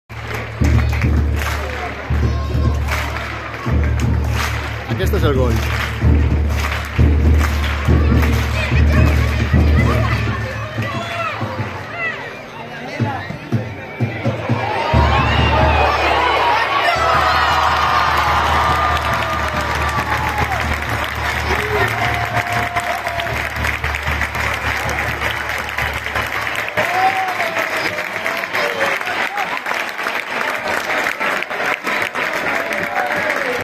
Gol de Geyse. FC Barcelona - Real Betis 7-0.
Primer gol del partit, Geyse Ferreira.
5/2/23